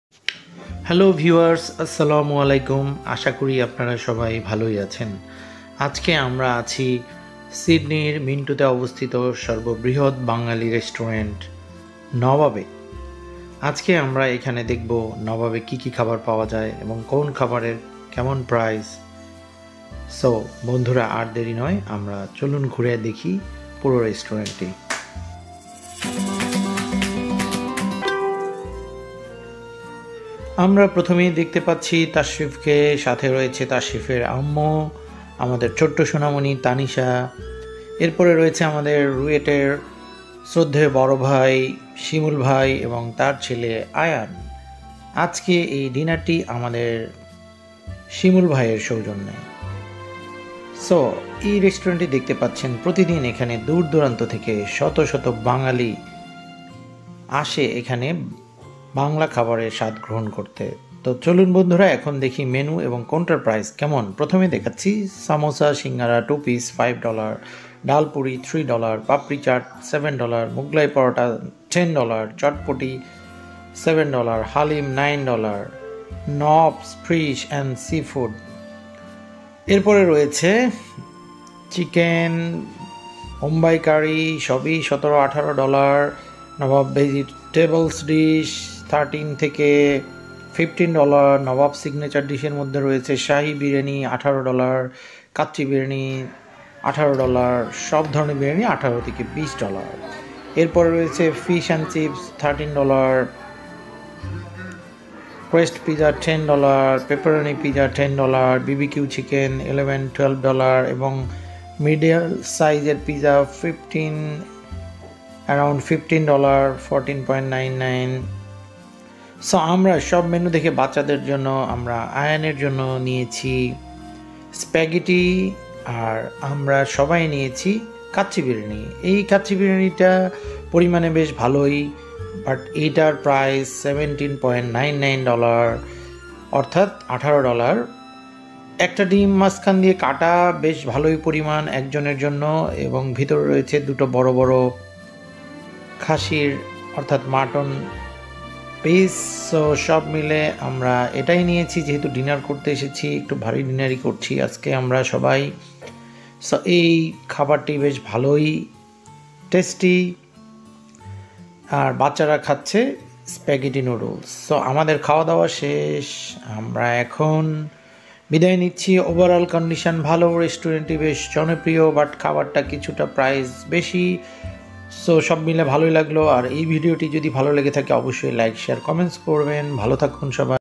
অষ্ট্রেলিয়ার বাঙালি রেষ্টুরেন্টে খাবারের দাম কেমন? Food Price at Bengali Restaurant in Australia
In this video we will show you the food Price at Bengali Restaurant in Sydney, Australia.